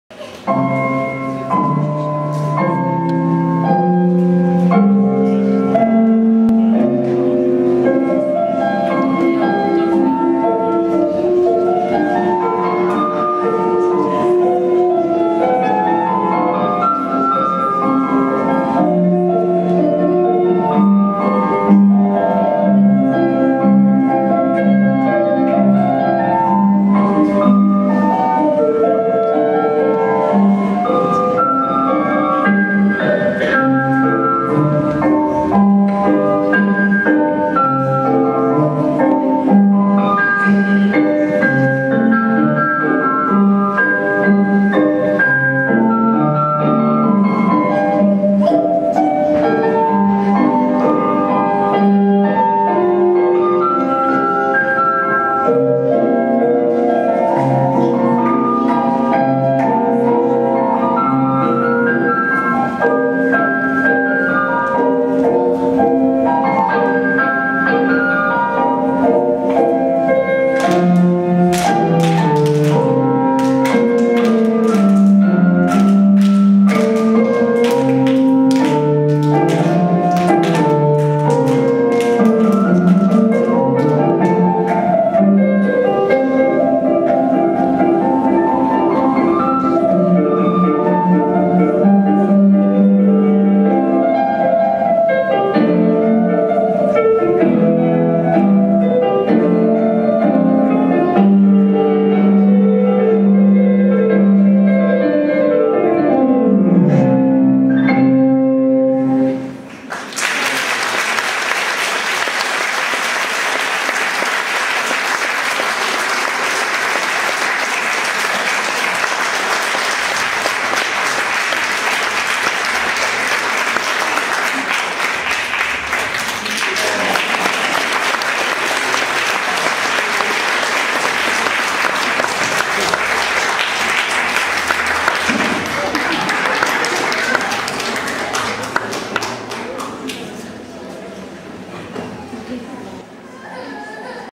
Taneční GALA 2008, Hudební koláž – Preparatoir
Taneční škola Jany Tomanové,
Taneční GALA 2008,
Hudební koláž – Preparatoir
Choreografie  – Jana Eriksson Tomanová
Tančí  – Justina Chadimová, Alice Holešovská, Natálie Koribská, Julie Krafková, Aneta Malinová, Alexandra Pertoldová, Adina Richterová, Anna Roušarová, Zuzana Synáčková, Eliška Votlučková, Daila Samar Lopezrios
(současně žákyně ZUŠ Charlotty Masarykové)